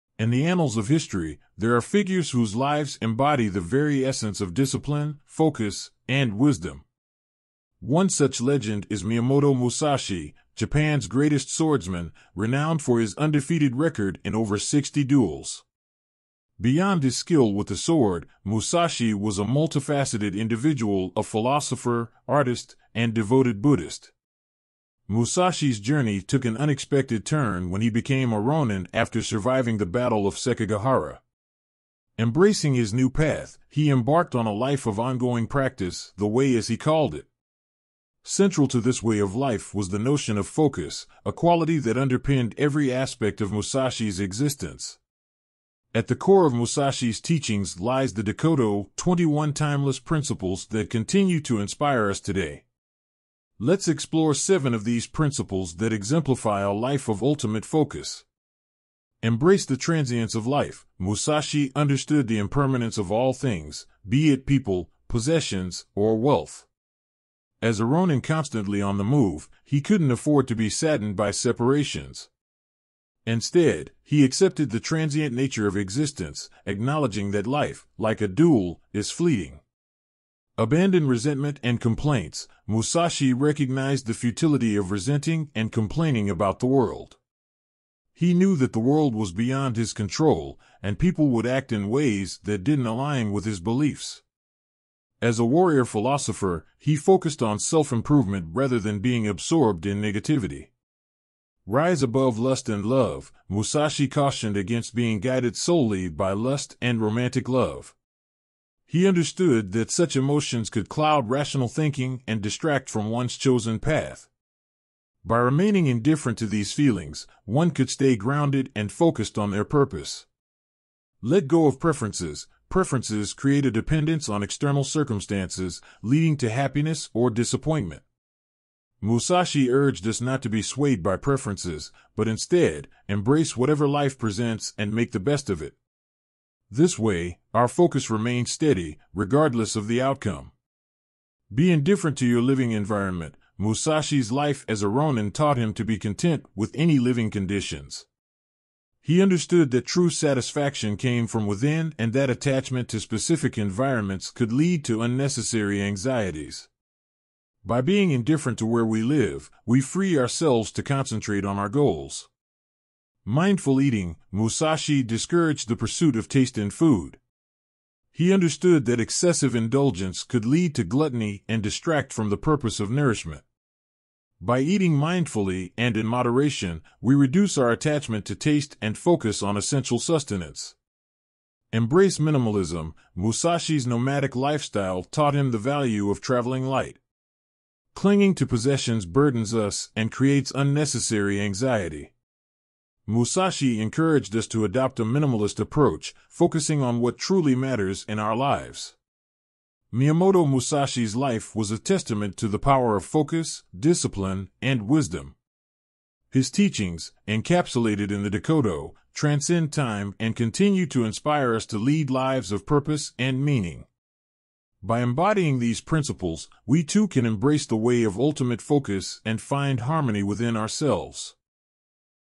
Miyamoto Musashi | A Life of Ultimate Focus
Discover the remarkable life of Miyamoto Musashi, Japan's greatest swordsman, and legendary samurai in 'Miyamoto Musashi | A Life of Ultimate Focus.' Follow his journey from undefeated duelist to enlightened philosopher, artist, and devoted Buddhist. Learn from his timeless principles in the Dokkōdō and find inspiration for living a life of purpose and focus.